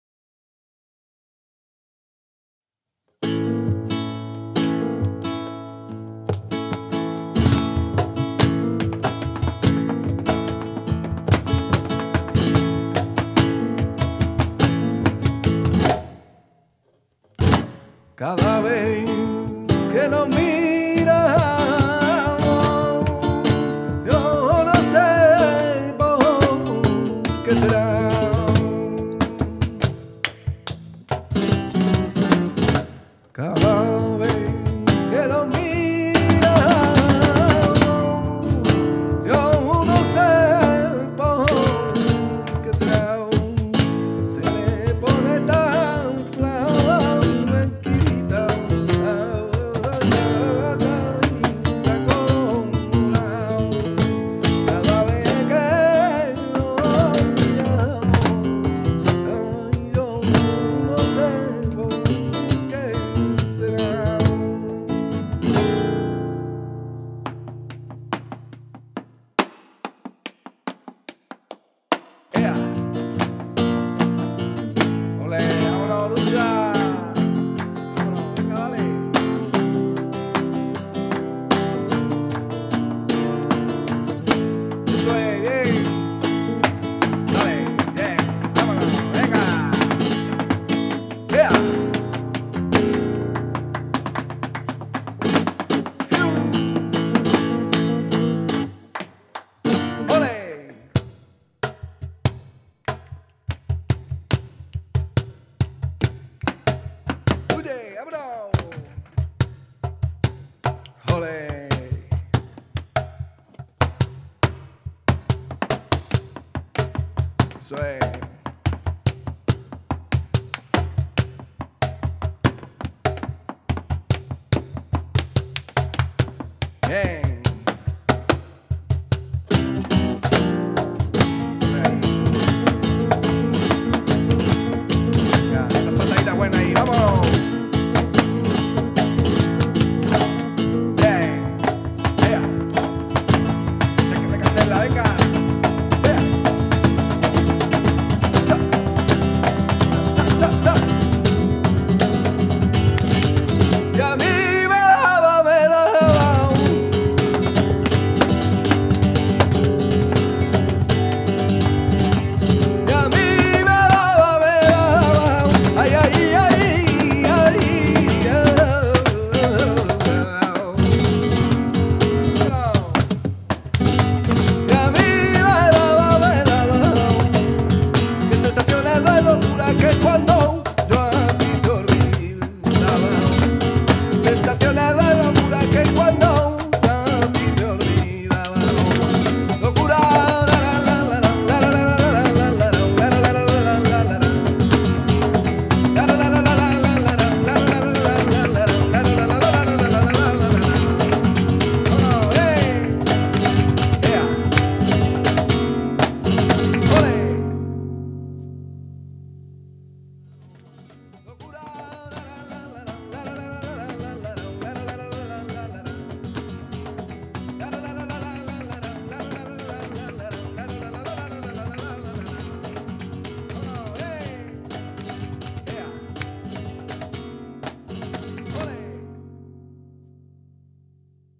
Solea por Bulerias
Dance - Luca Szegőfi
Music - Jorge Díez Aragón vocal, Péter Oravecz guitar, Gergely Nyári cajon
Dress - Adrienne Körtvély
Make up - Adrienn Safarcsik
Photographer - Márton Kovács
Cut - Kornél Viktorin
Sound - MI Studio Miklós Szabó